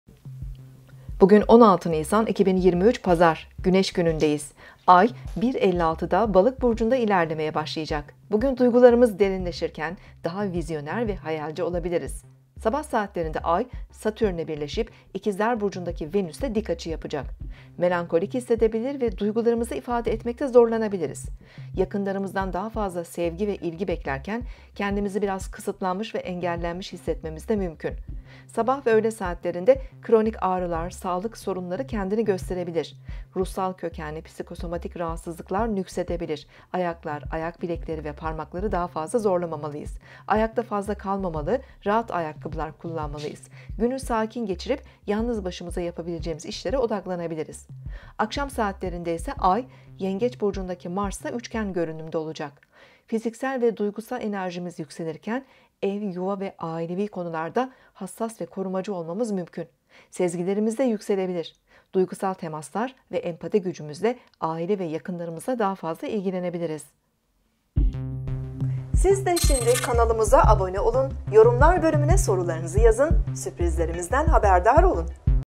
DUYGUSAL VE HAYALCİYİZ. 16 Nisan 2023 Pazar, Günlük burç yorumu, Astrolog Demet Baltacı astroloji
Bugün 16 Nisan 2023 pazar güneş günündeyiz ay 1.56'da balık burcunda ilerlemeye başlayacak bugün duygularımız derinleşirken daha vizyoner ve Hayalci olabiliriz Sabah saatlerinde ay satürne birleşip ikizler burcundaki Venüs de dik açı yapacak Melankolik hissedebilir ve duygularımızı ifade etmekte zorlanabiliriz yakınlarımızdan daha fazla sevgi ve ilgi Beklerken kendimizi biraz kısıtlanmış ve engellenmiş hissetmemiz de mümkün sabah ve öğle saatlerinde kronik ağrılar sağlık sorunları kendini gösterebilir ruhsal kökenli psikosomatik rahatsızlıklar Nüks edebilir ayaklar Ayak bilekleri ve parmakları daha fazla zorlamamalıyız ayakta fazla kalmamalı rahat ayakkabılar kullanmalıyız günün sakin geçirip yalnız başımıza yapabileceğimiz işlere odaklanabiliriz akşam saatlerinde ise ay yengeç burcundaki Mars'a üçgen görünümde olacak fiziksel ve duygusal enerjimiz yükselirken DUYGUSAL VE HAYALCİYİZ. 16 Nisan 2023 Pazar, Günlük burç yorumu, Astrolog Demet Baltacı astroloji